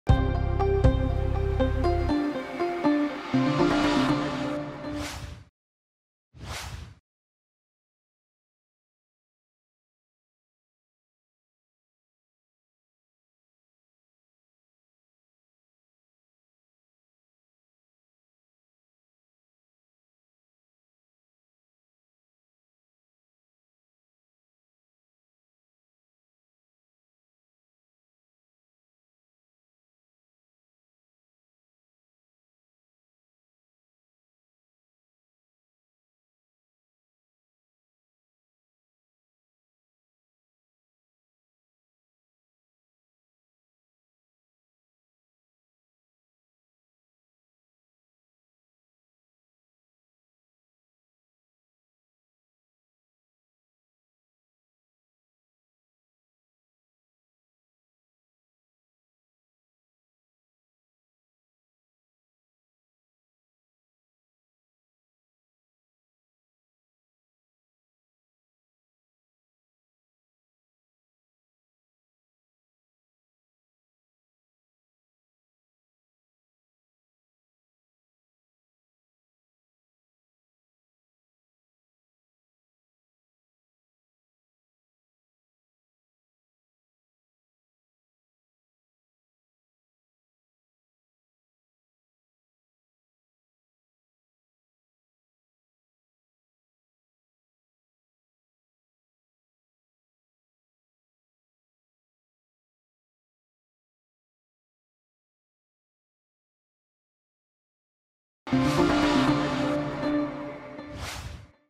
Մարիուպոլի ավերված թատրոնը. ռմբակոծության հետևանքով 300 մարդ է զոհվել
1in.am Armenian News & Analyses.

Drone footage shows destroyed theatre in besieged Mariupol, Ukraine